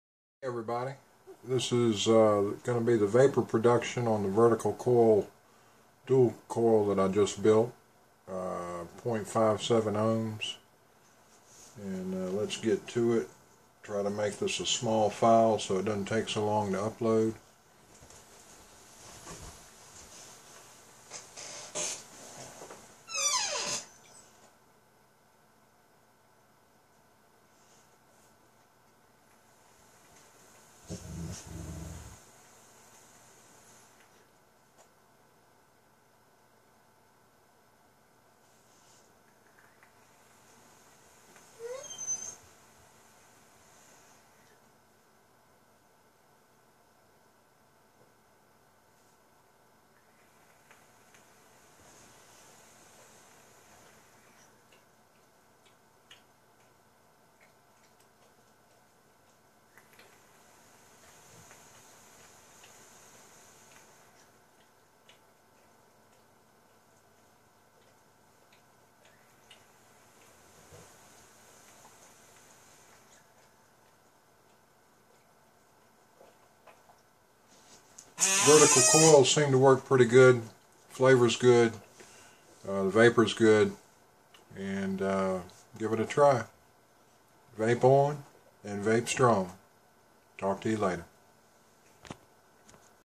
Dual vertical coil, 28ga, 6 wraps at .57 ohms. Vapor Production
Vapor production with dual vertical coils, 28ga, 6 wraps at .57 ohms with cotton on an FT Chimbus with two 3/32" air holes. Good flavor and cool vapor. Better than horizontal coils, better air circulation.
for more information about using cotton wicks in all types of atomizers.